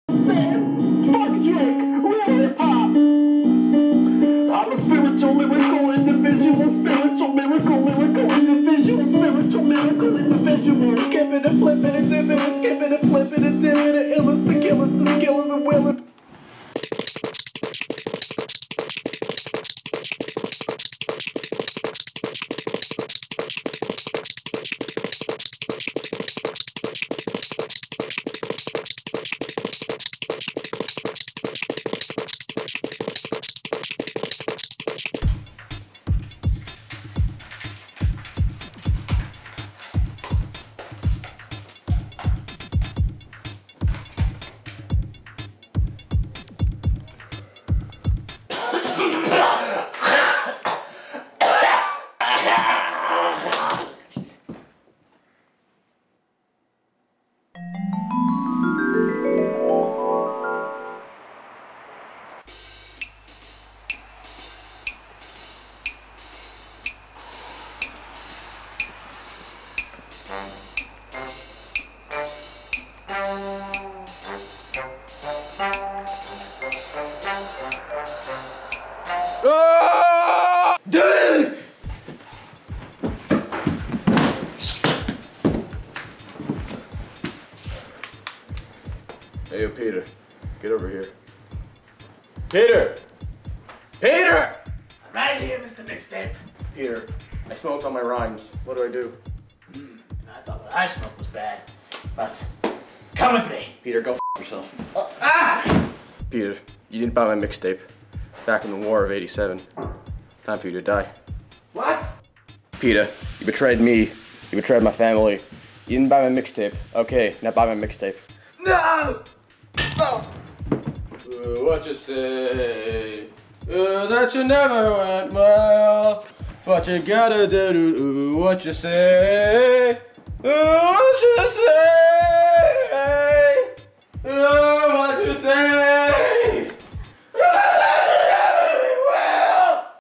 The fall of Mista Mixtape
Mista Mixtape is back for yet another trilogy! This time, after latterly smoking all his rhymes, he's run out of ideas for new mix tapes and now must struggle to break out of his creative block.

Starring all 3 J's in Team 3 J: "Jonathan Hersey (Jonathan10597)" "John Farraro (Hydra Studios 16)" and  "Justin Woods (CGhorizons)".